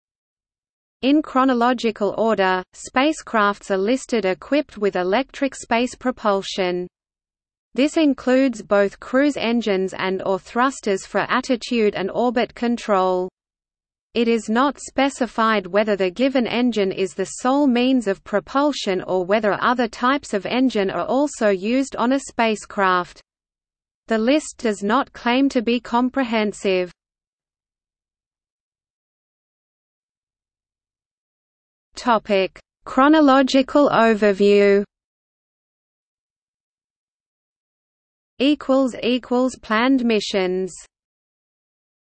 List of spacecraft with electric propulsion | Wikipedia audio article
00:00:28 Chronological overview

- Socrates

SUMMARY
In chronological order, spacecrafts are listed equipped with electric space propulsion. This includes both cruise engines and/or thrusters for attitude and orbit control. It is not specified whether the given engine is the sole means of propulsion or whether other types of engine are also used on a spacecraft. The list does not claim to be comprehensive.